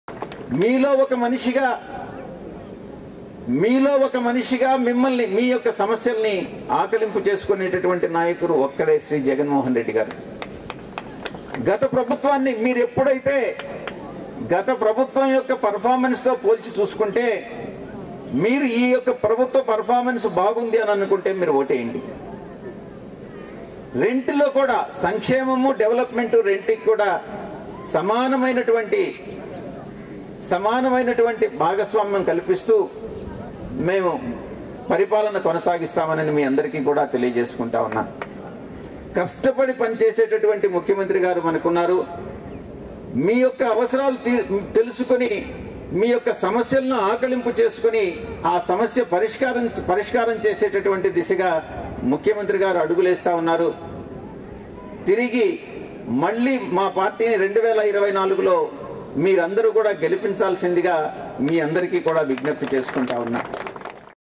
మనలో ఒకడు సీఎం శ్రీ వైఎస్ జగన్ గారు
ప్రజల సమస్యలు తెలిసిన నాయకుడు, మనలో ఒకడు గౌరవ ముఖ్యమంత్రి శ్రీ వైఎస్ జగన్ గారు. సంక్షేమానికి, అభివృద్ధికి సమప్రాధాన్యమిస్తూ పరిపాలన అందిస్తున్నారు. వచ్చే ఎన్నికల్లోనూ వైఎస్సార్ కాంగ్రెస్ పార్టీని ఇలాగే ఆదరించాలని విజ్ఞప్తి చేస్తున్నా.